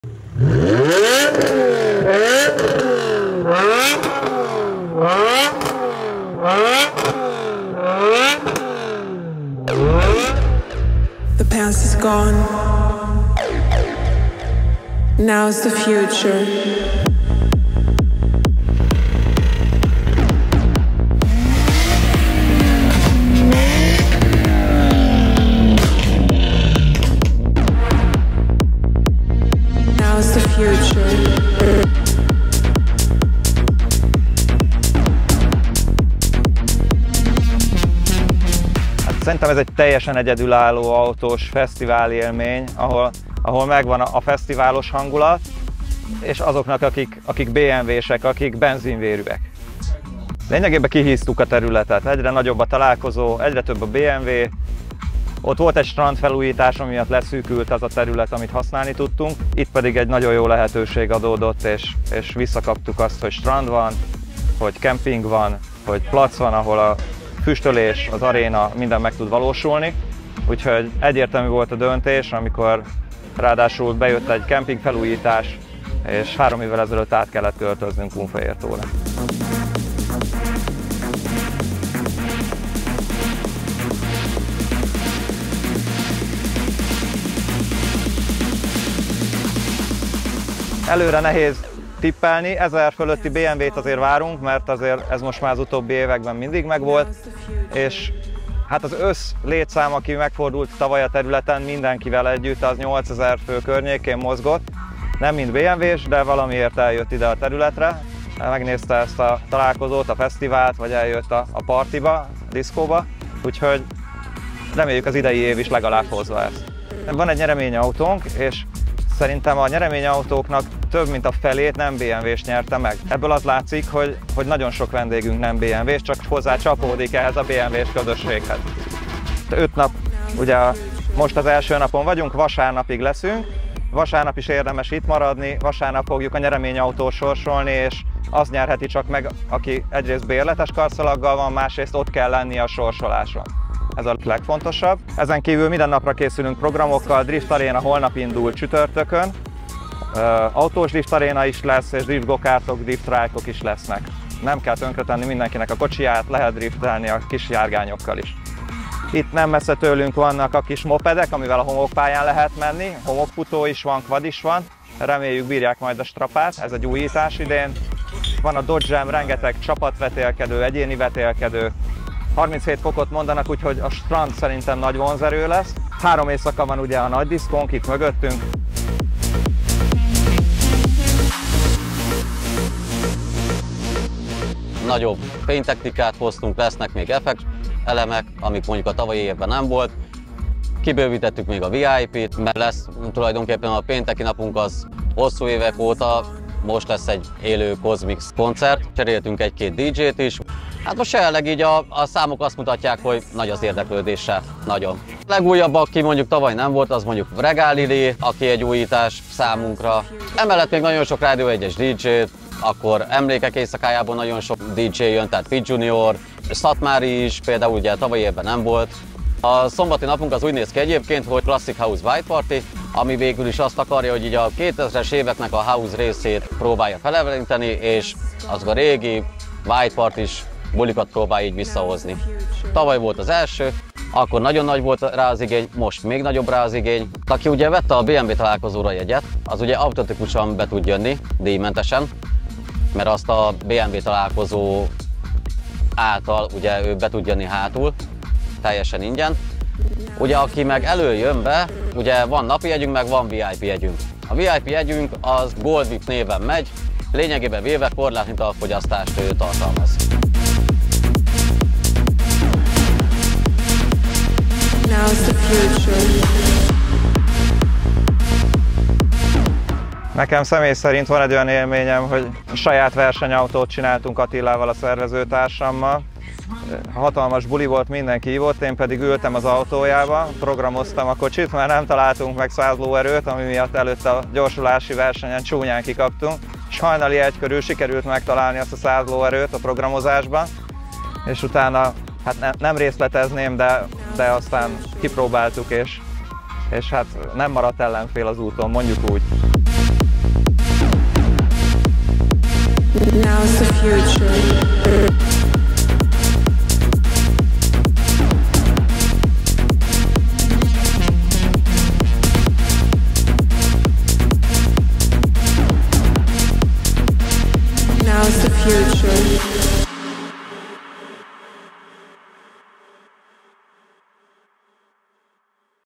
Szecseivel és Regán Lilivel rajtol ma a kunfehértói őrület
...ahová a szegedi BMW klub egy guruló kádban érkezett, és ez komoly!